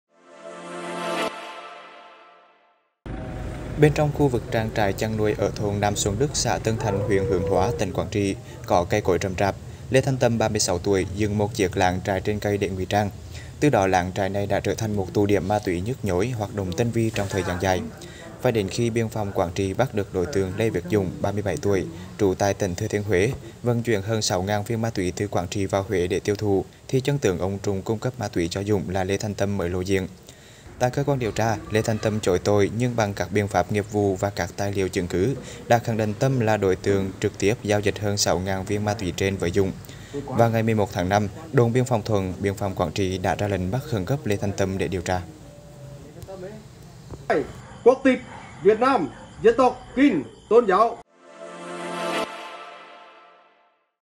Bí mật trong chiếc lán dựng trên cây ở vùng biên giới Việt - Lào | Báo Lao Động
BÁO LAO ĐỘNG | Lợi dụng địa hình cây cối rậm rạp tại một trang trại ở khu vực biên giới tỉnh Quảng Trị, người đàn ông dựng một chiếc lán ở trên cây để làm tụ điểm sử dụng, mua bán ma túy.

* Đường dây nóng: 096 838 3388